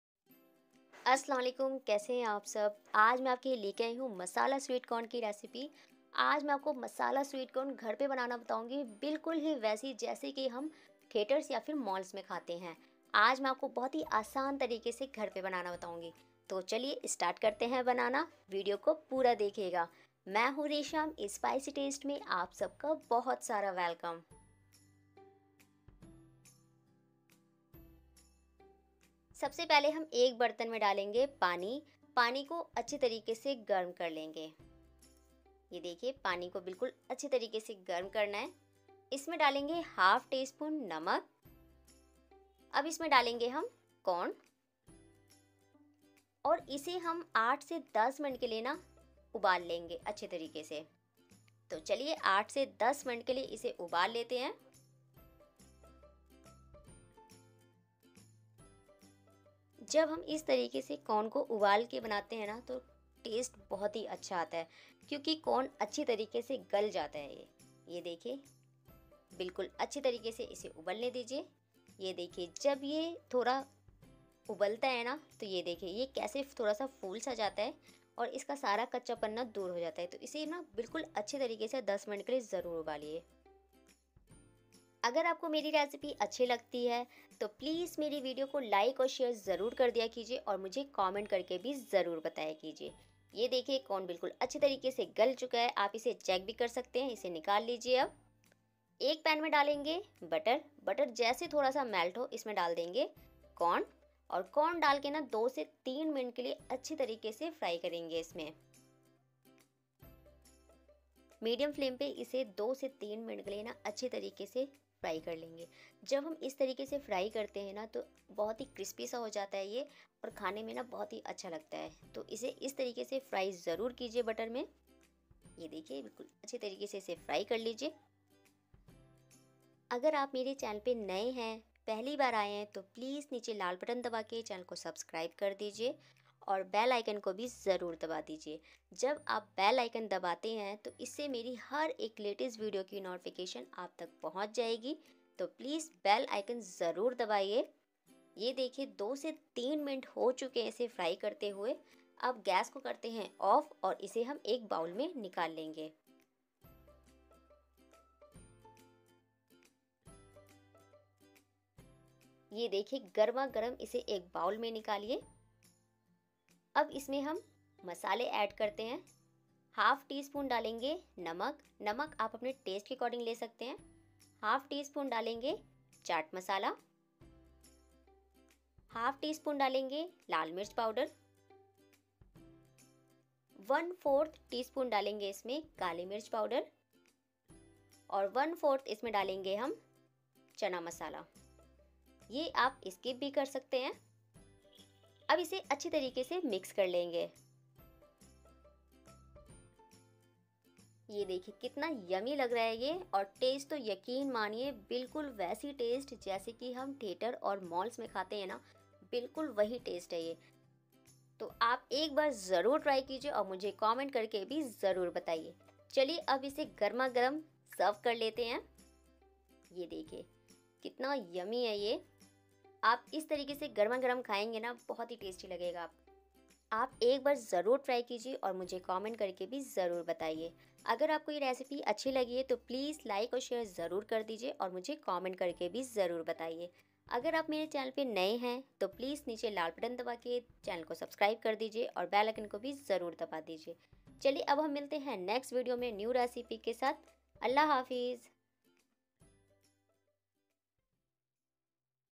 Masala Sweet Corn Recipe | How to make Sweet Corn
masala Sweet Corn..

corn
Butter
Red chilli
black pepper
Salt
chaat masala
chana masala

i use _---
ring light
kitchen item

Folloe me;